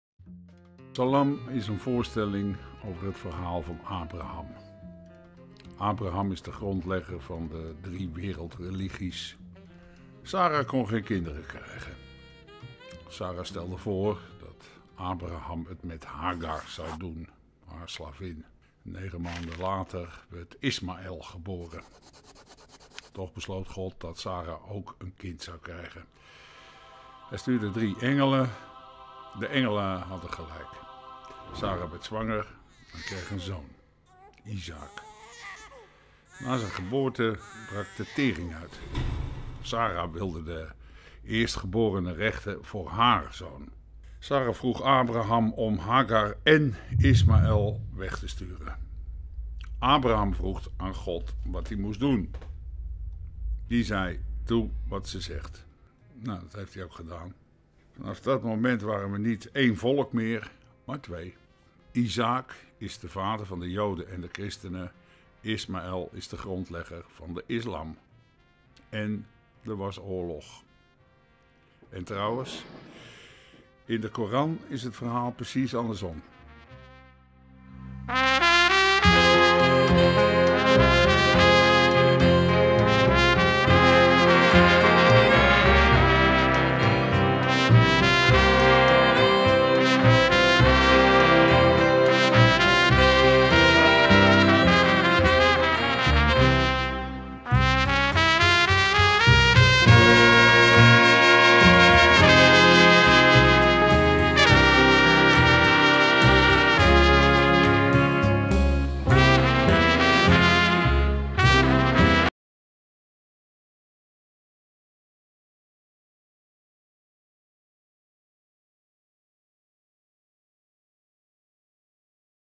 Teaser Salam - Language No Problem, English subtitles
Noord Nederlands Toneel + Club Guy & Roni +Asko|Schönbergs K[h]AOS SALAM Major, religious and societal conflicts are brought back to human stories All major stories develop from a small story. And practically all world conflicts from disappointment or pain. Now that the tensions between the Western and the Arabic world, between Islam, Judaism and Christianity keep mounting more and more, the Noord Nederlands Toneel (NNT), Club Guy & Roni and Asko|Schönberg take the theatregoer back to where it all started: a father who does not know how to show his fatherly love and two brothers who are driven away from each other. It is the story of Abraham, played by Jack Wouterse. In this play, director Guy Weizman brings major, religious and societal conflicts back to human stories about love, regret, jealousy and pain.

CAST & CREATIVES:
Direction: Guy Weizman Script: Fikry El Azzouzi Choreography: Roni Haver, Composers: Genevieve Murphy, Florian de Backere, Luke Deane, Yannis Kyriakides, Abderrahim Semlali, coaching composers: David Dramm, Music: Asko|Schönberg’s K[h]AOS
Cast: Jack Wouterse, Bien de Moor, Bram van der Heijden, Senna Gourdou, Veerle van Overloop, Mohammed Azaay, Igor Podsiadly, Angela Herenda, Adam Peterson, Sofiko Nachkebiya en Camilo Miguel Chapela, Harold Luya, Dramaturgy: Robbert van Heuven Images: Ascon de Nijs